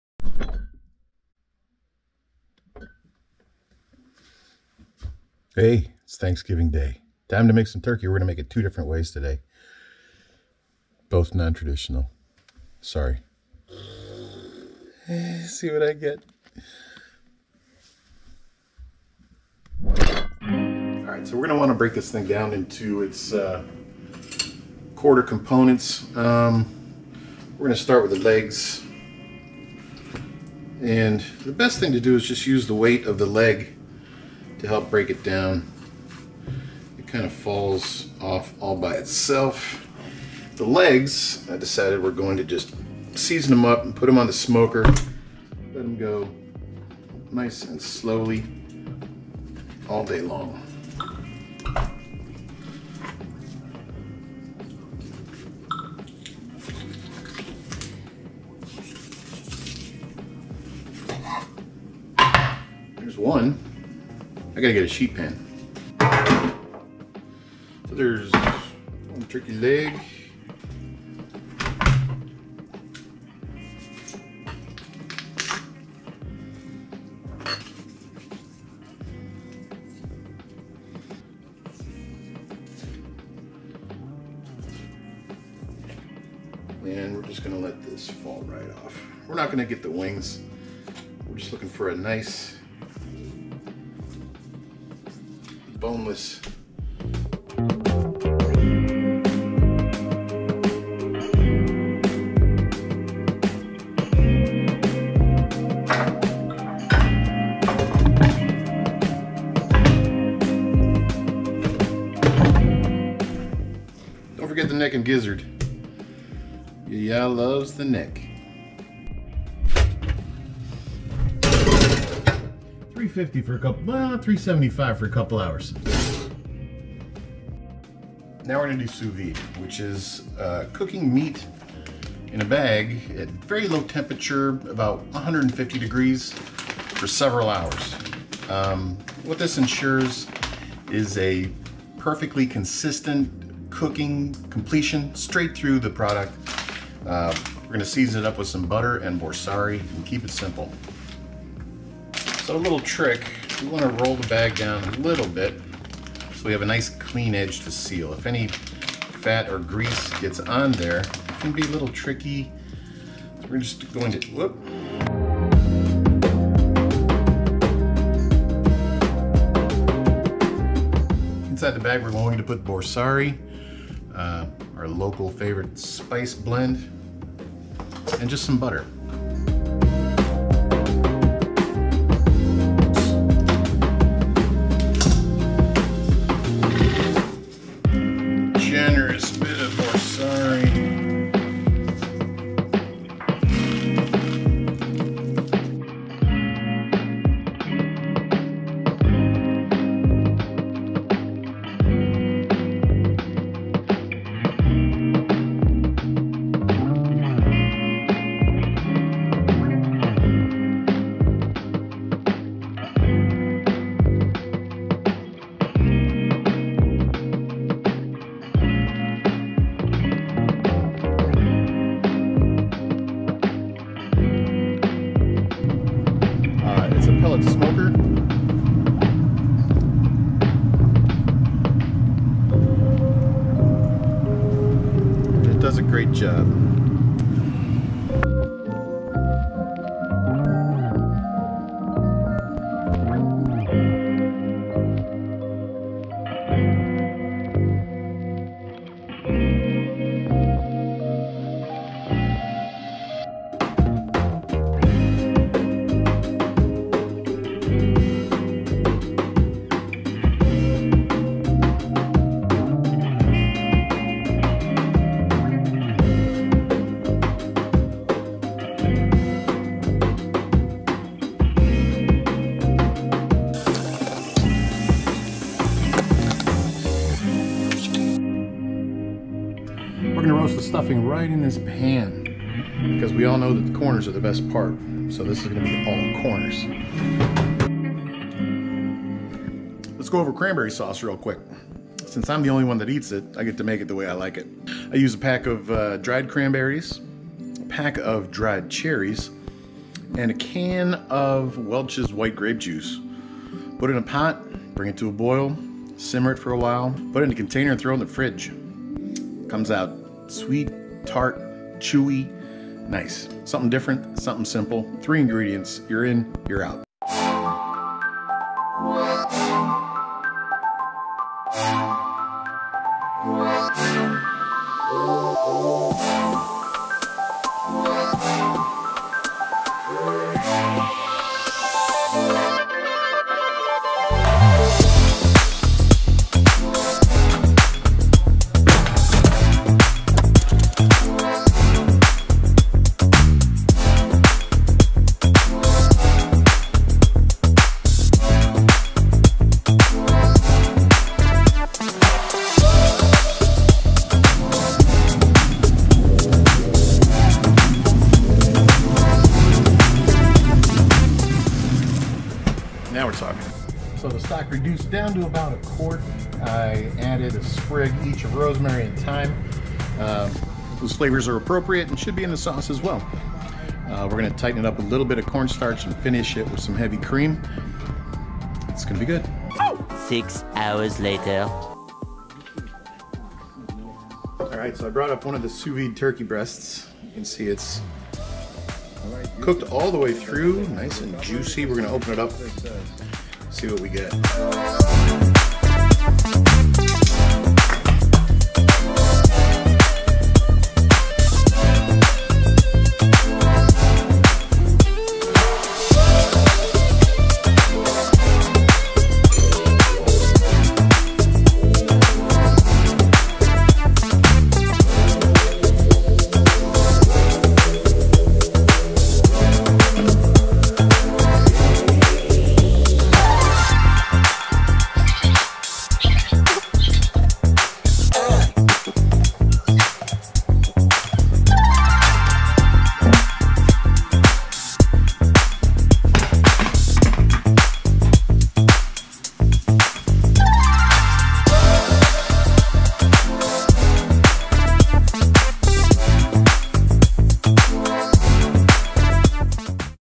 Turkey two ways! Sous Vide and Smoked!
I take one bird and cook it two ways!